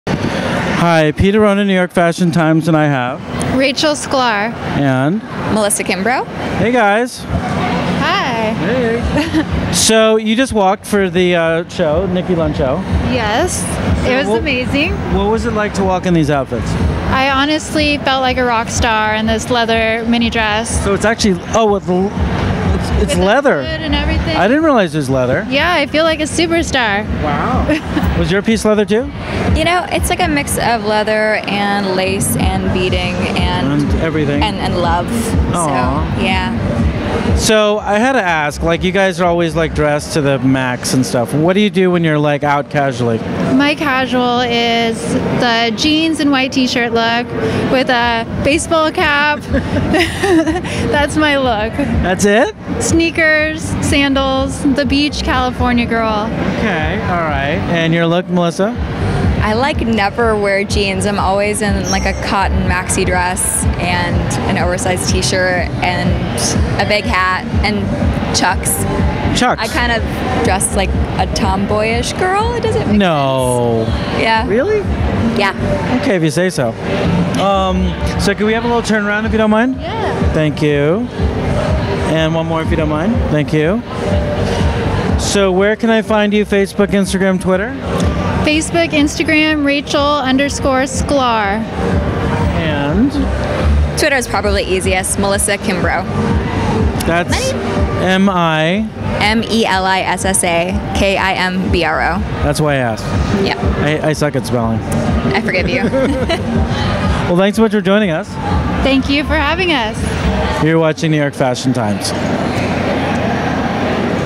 Models Rachel Sklar and Melissa Kimbro backstage after the Nikki Lund Show
it helps us get more videos to you.